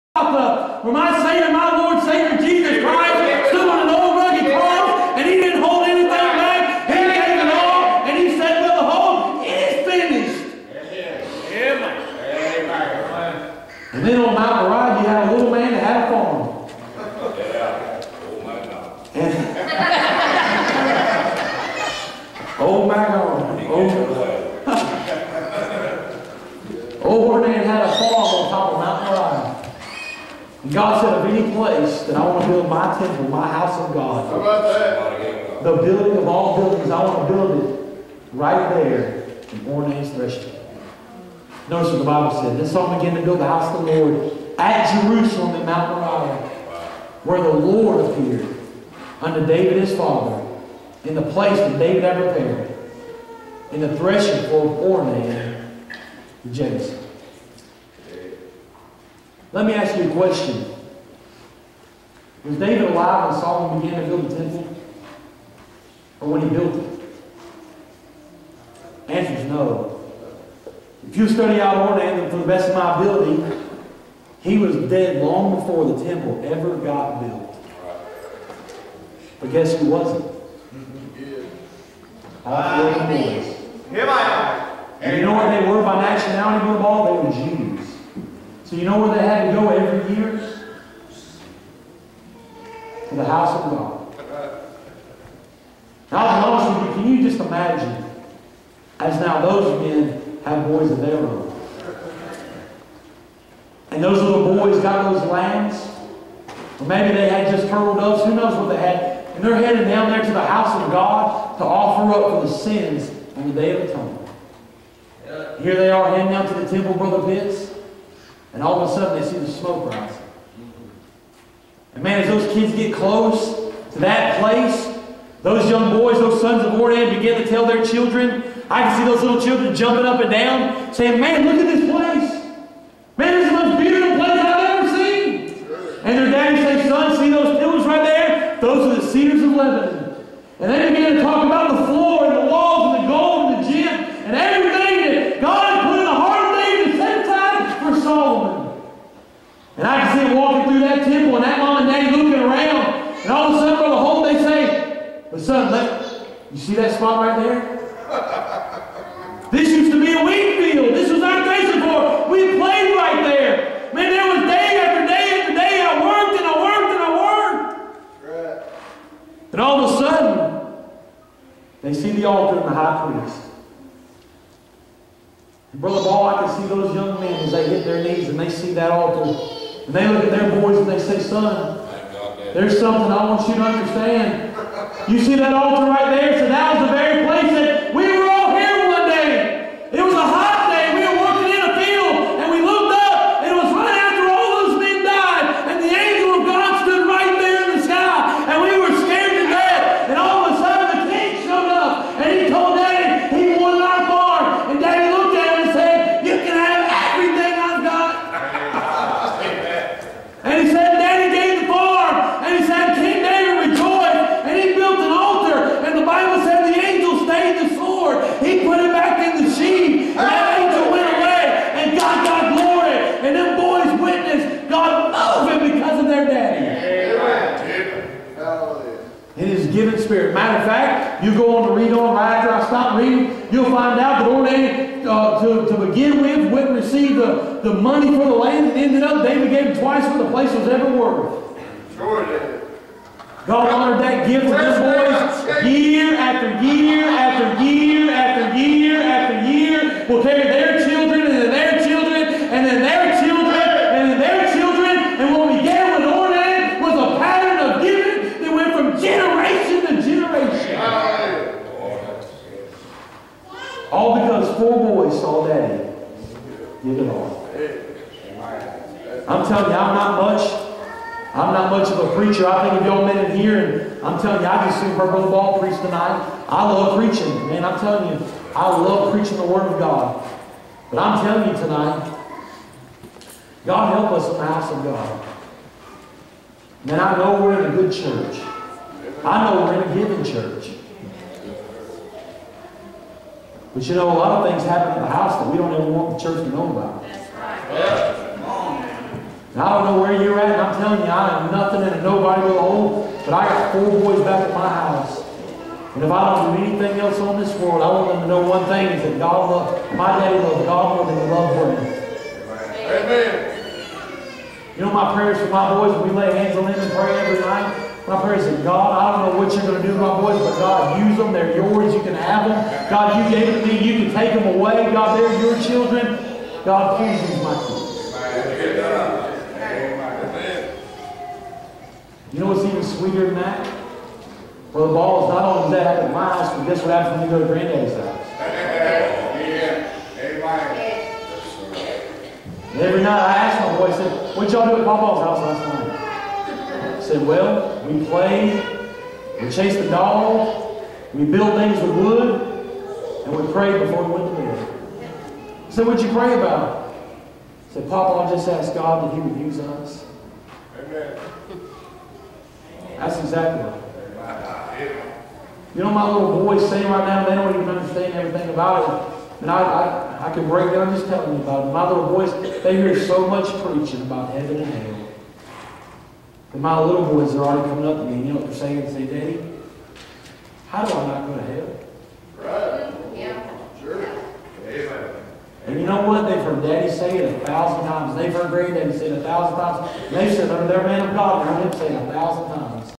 PASTOR ERIC LEE 8/9
PASTOR ERIC LEE PREACHING A MESSAGE AT THE 16TH ANNUAL FAITH BAPTIST CHURCH MISSIONS REVIVAL PRIMROSE, GA

MESSAGE: I WAS THERE WHEN DADDY GAVE IT ALL

PASTOR BILLY BALL